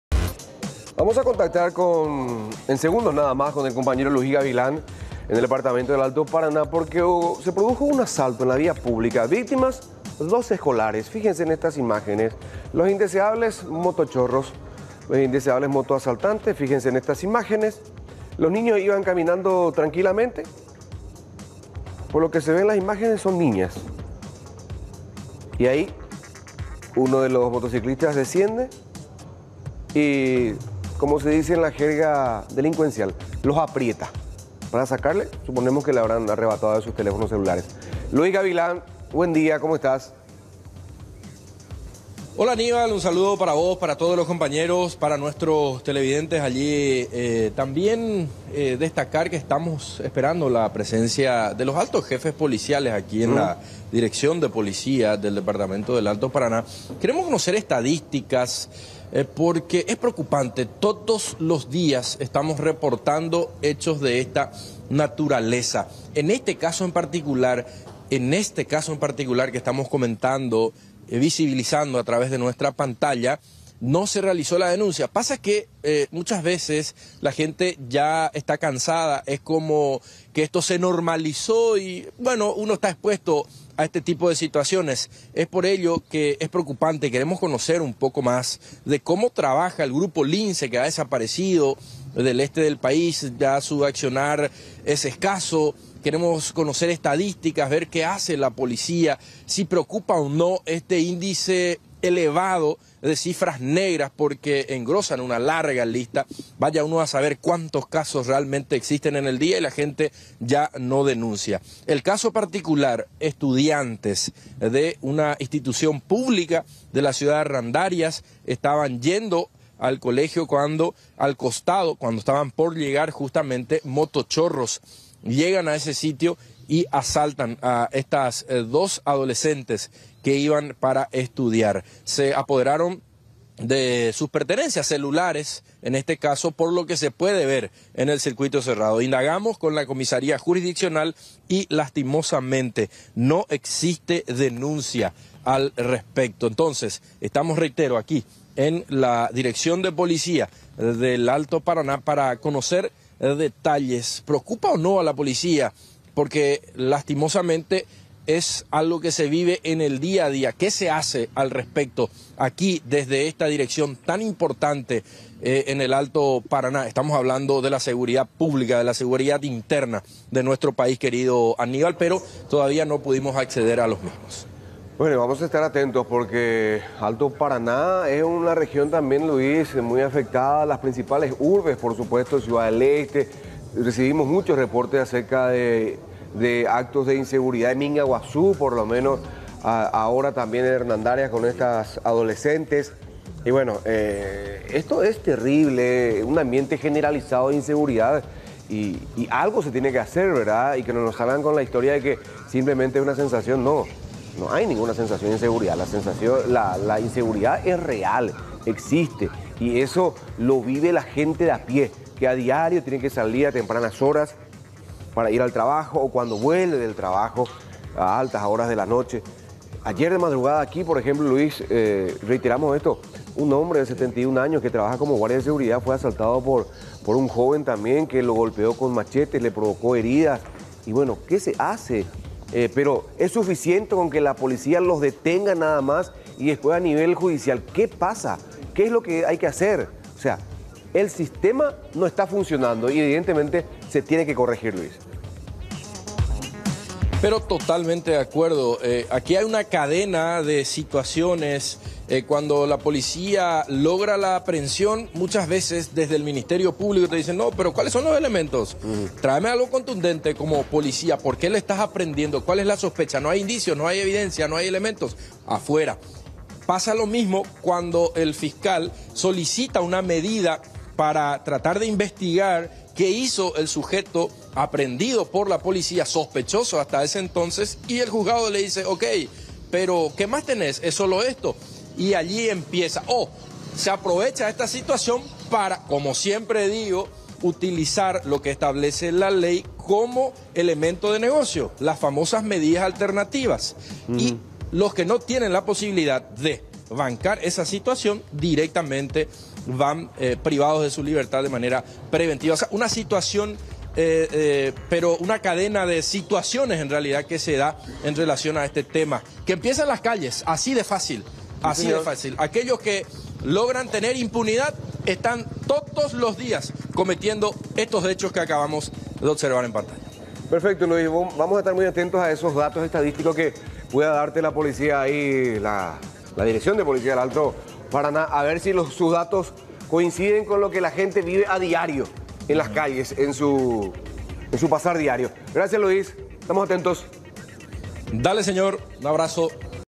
Hernandarias: Les robaron hasta los útiles escolares
Estudiantes fueron asaltados en Hernandarias, donde los ladrones le dejaron hasta sin sus útiles escolares.